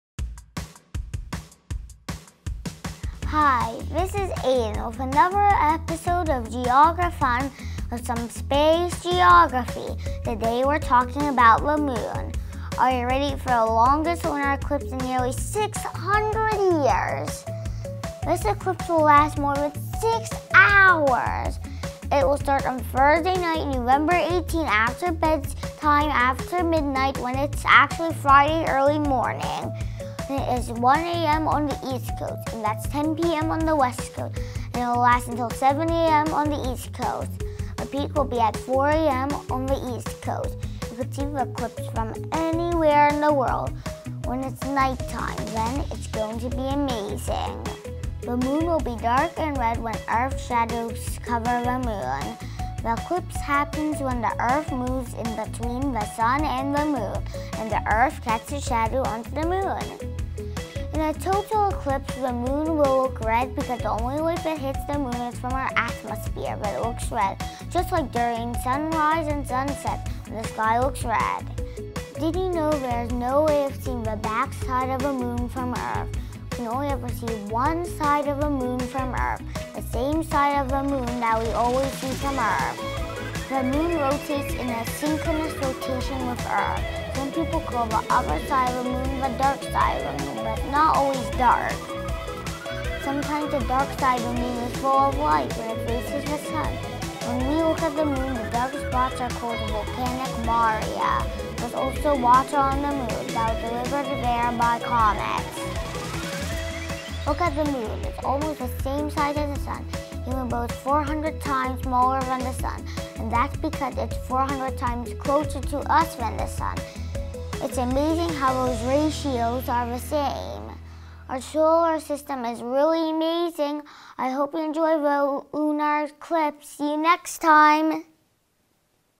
Lunar Eclipse - When to Watch and All About the Eclipse - Geografun
Are you ready to watch the lunar eclipse on November 18th? Check out when to watch and all about the lunar eclipse with 7-year-old Aiden. The Earth will cast its shadow on the moon late night November 18.